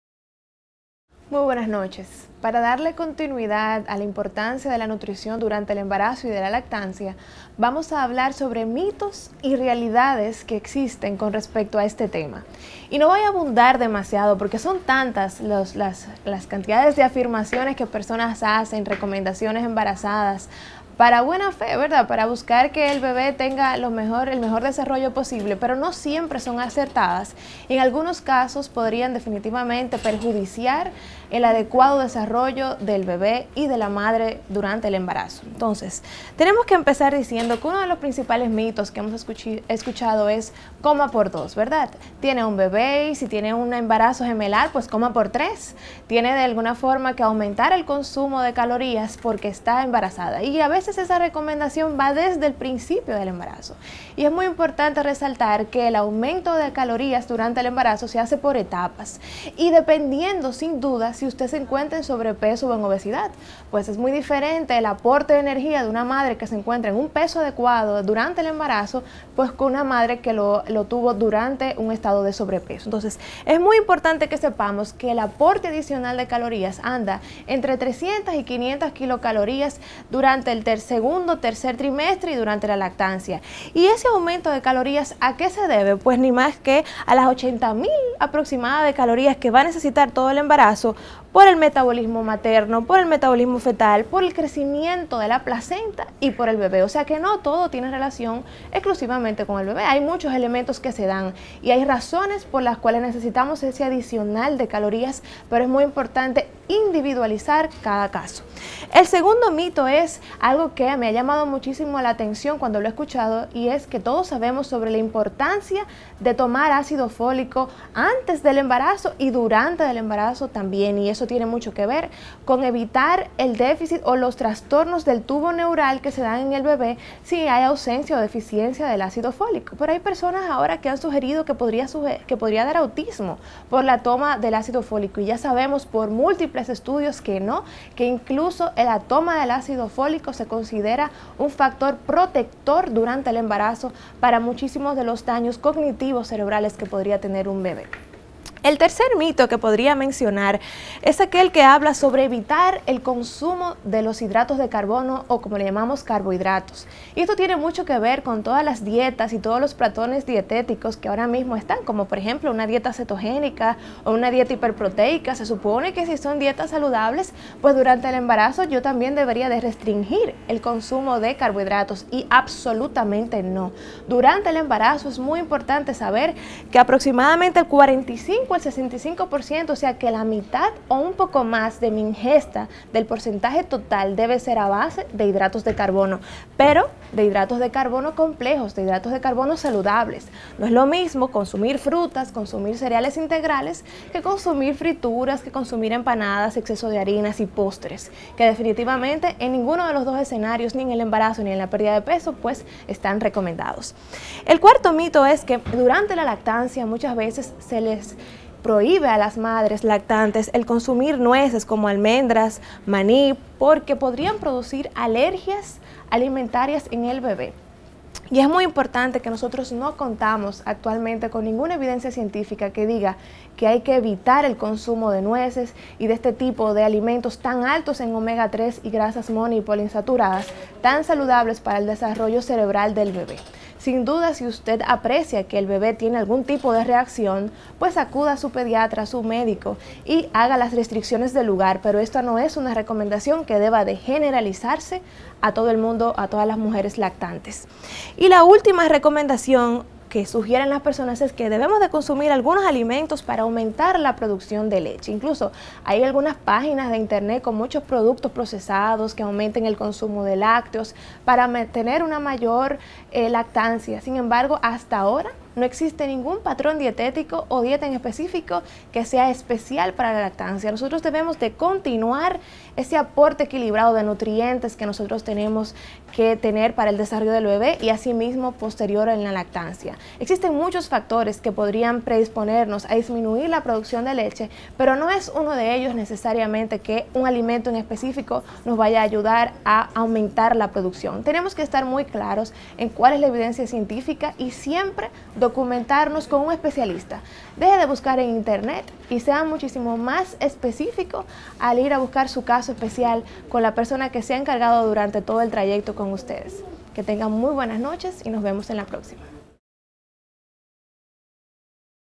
Importancia de la adecuada alimentación durante el embarazo y la lactancia
SANTO DOMINGO, República Dominicana.- Es importante darle continuidad a la adecuada alimentación durante el embarazo y la lactancia, debido a que existen muchos mitos y realidades en cuanto a este tema, para eso hablaremos a continuación con la nutrióloga Erika Pérez.